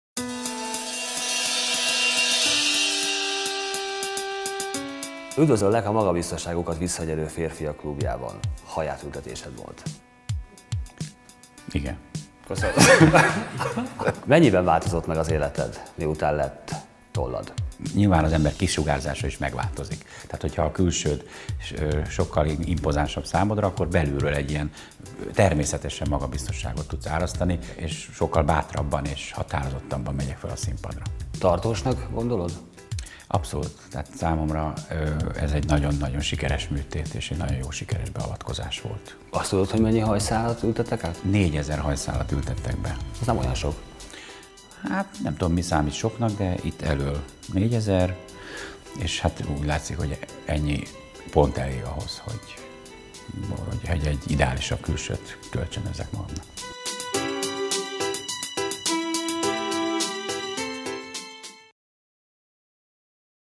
Éliás Jr. hajbeültetése - "Sokkal bátrabban és határozotabban megyek fel a színpadra!"
Éliás Jr.  Artisjus-díjas magyar zenész, énekes hajbeültetése , melyet a HairHungary klinikán végeztek el. Tordai István autós műsorvezető, - akinek szintén volt már hajátültetése, mint sorstárs kérdezi az eredményességről.